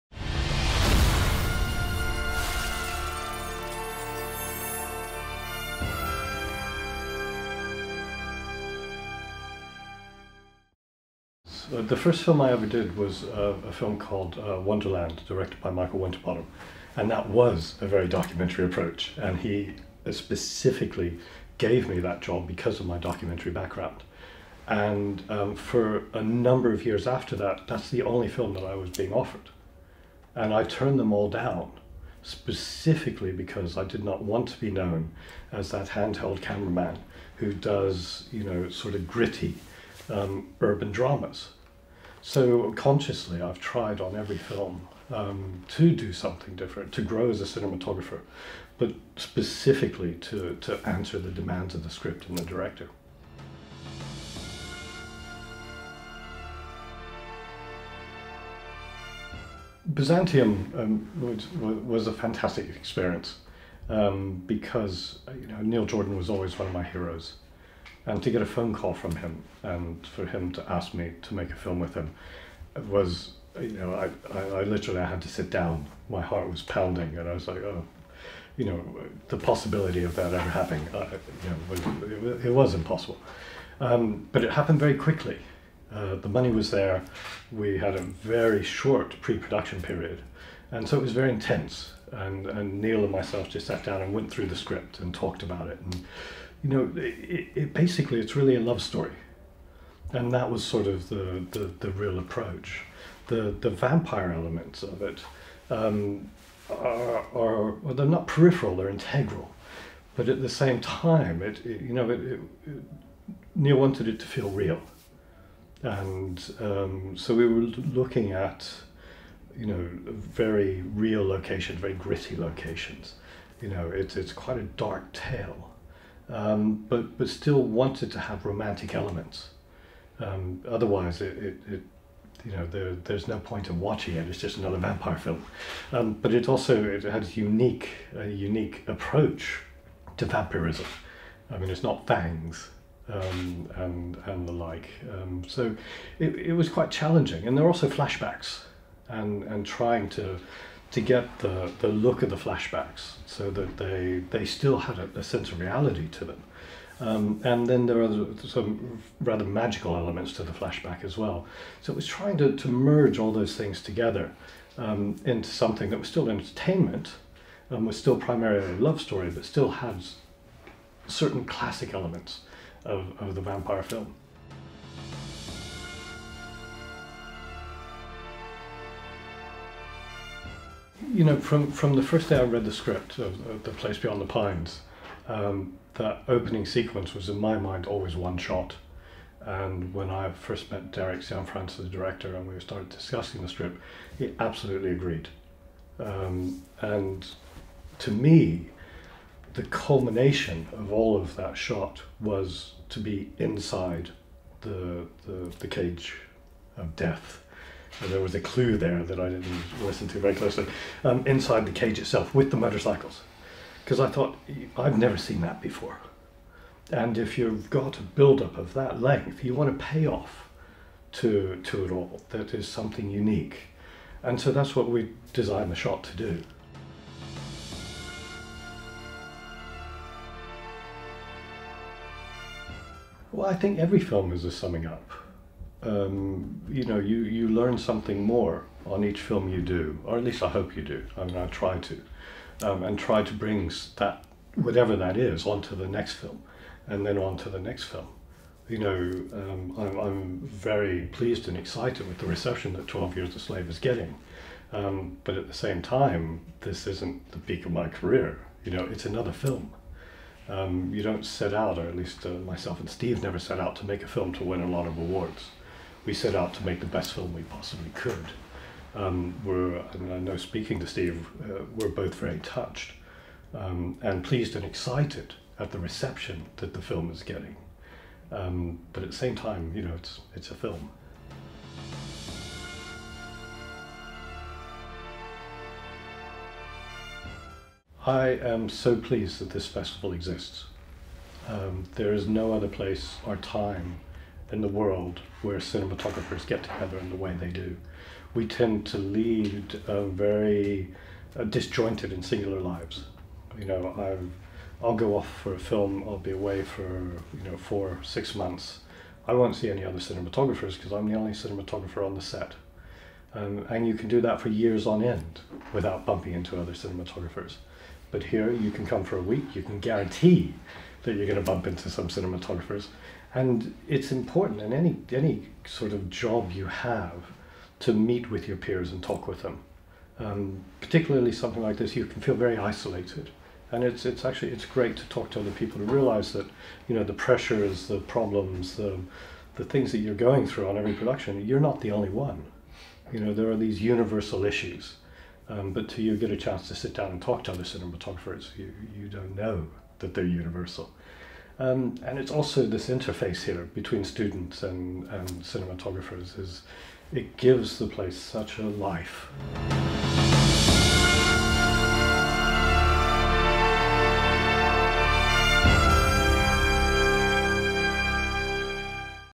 Camerimage Sean Bobbitt interview
Darek Kuzma talks with Sean Bobbitt, the cinematographer behind "12 Years a Slave", "Byzantium" and "The Place Beyond the Pines", among many other.

The crew behind the interview:
Cinematographer: Krzysiek Wyszomirski
Editor: Piotr Bednarski
Translation: Kasia Kuźma
Host: Darek Kuźma